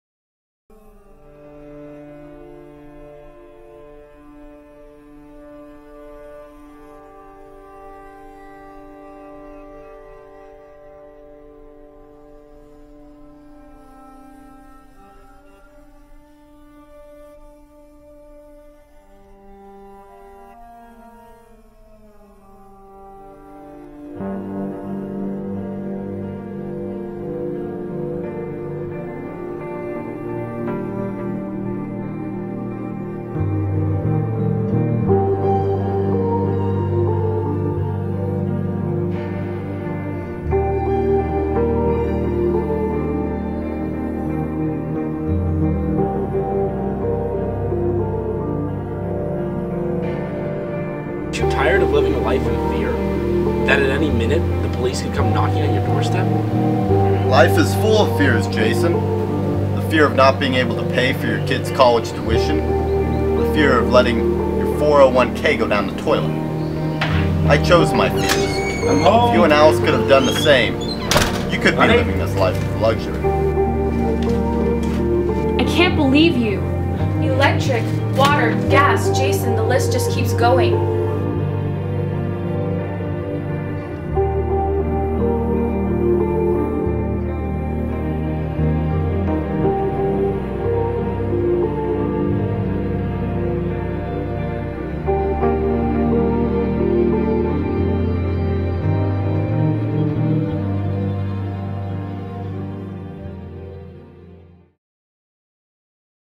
Goodnight Alice Teaser
A teaser for the Epitome Production of Goodnight Alice, written directed and starring Colin Gray.
Coming to OHS Theater's